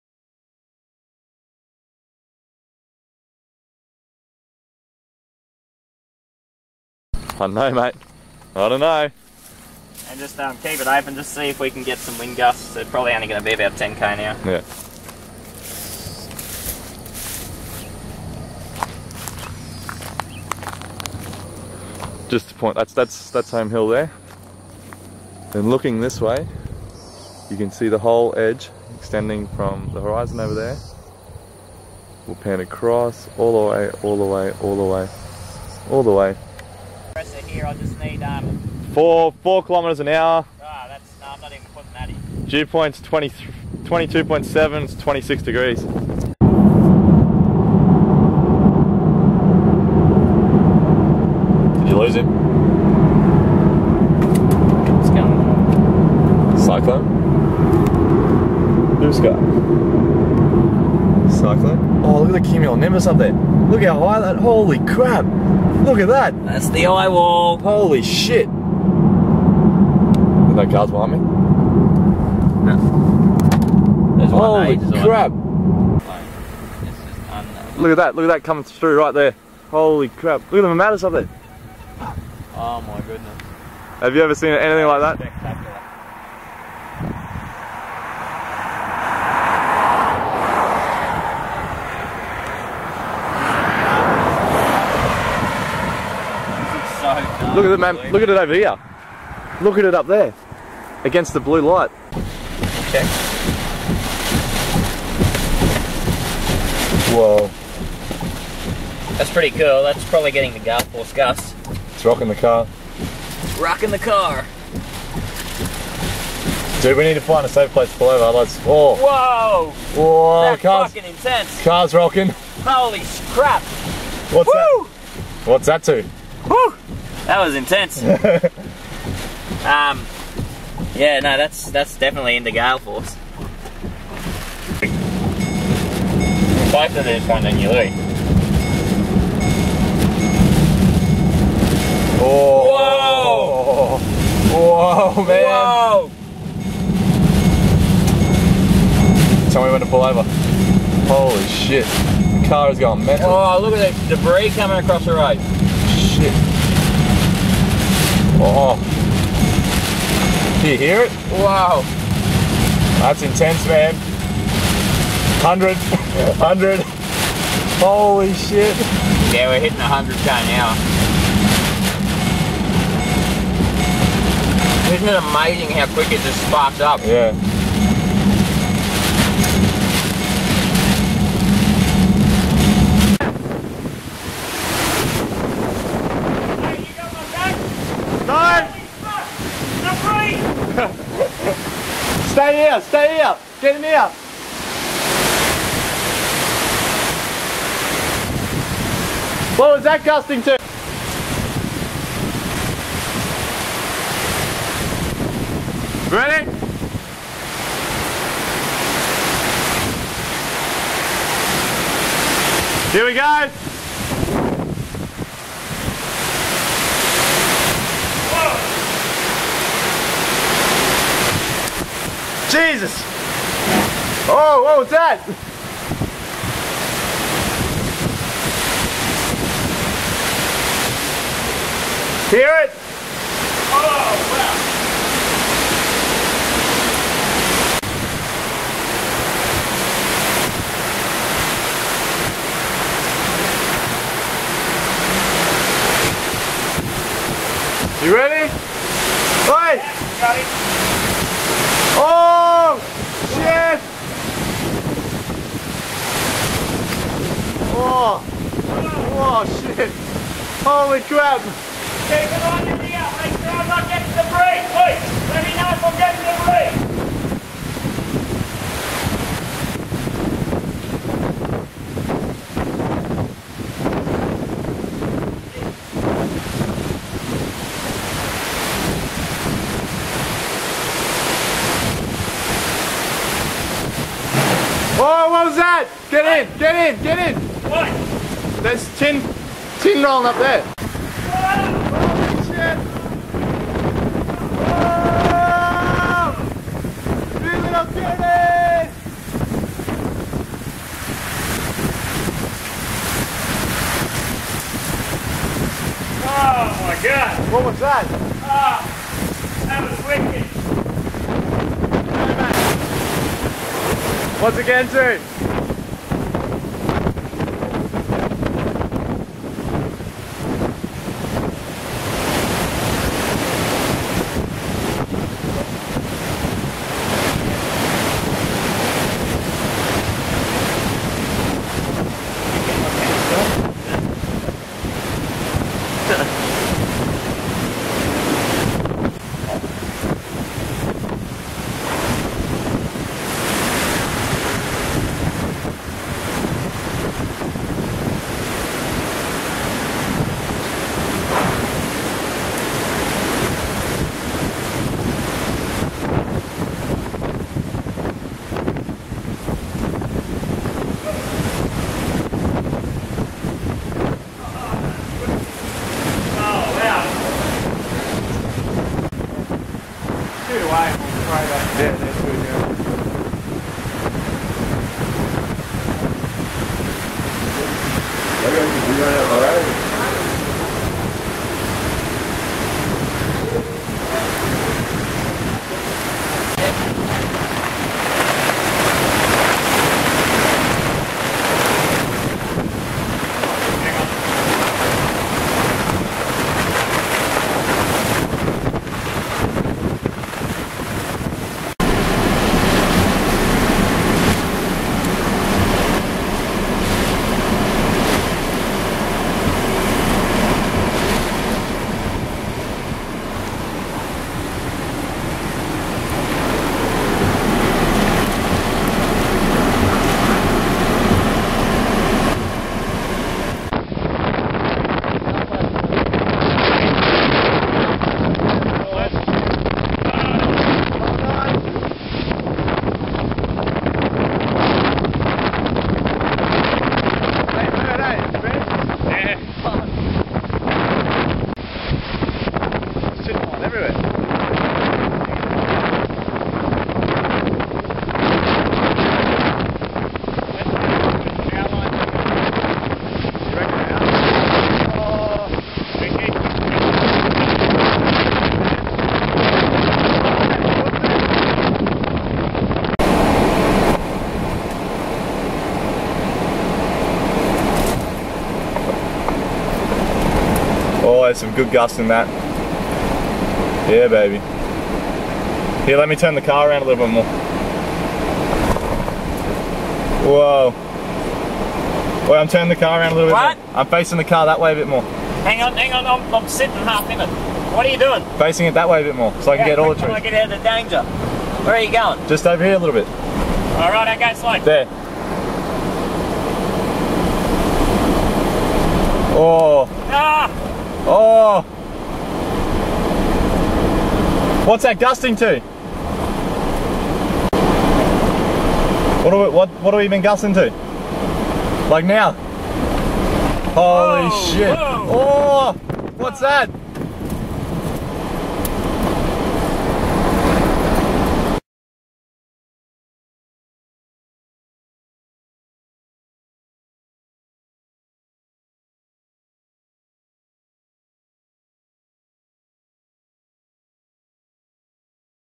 Tropical Cyclone Anthony Chase
We intercepted Cyclone Anthony at Bowen Queensland. We recorded a max gust of 104kmhr during the passage of the system, however we estimate winds to have been closer to 115-125kmhr. The buisness district that we were sheltering in was stopping the strongest gusts from occuring near us.